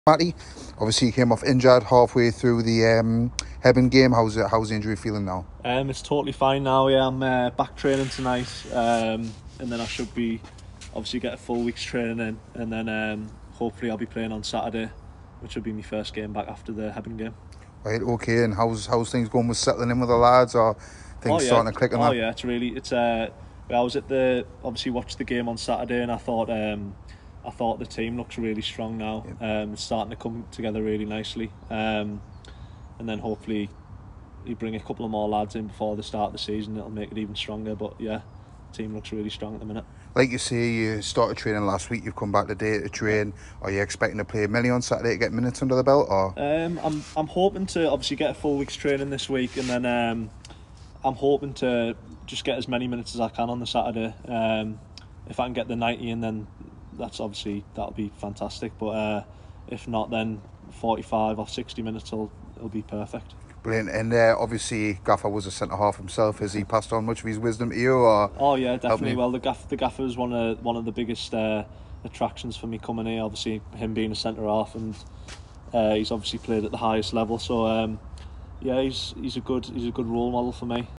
INTERVIEW | Matthew Elsdon provides injury update
The former Middlesbrough defender hasn't played since the opening game of pre-season, however is hoping for minutes this weekend.